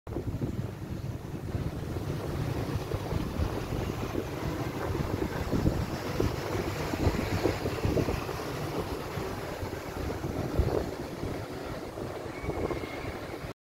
Mekong River. Town My Tho. Travel Vietnam.
Rauhallinen pikkukaupunki suurjoen päähaaran rannalla. My Tho is a small town by the river.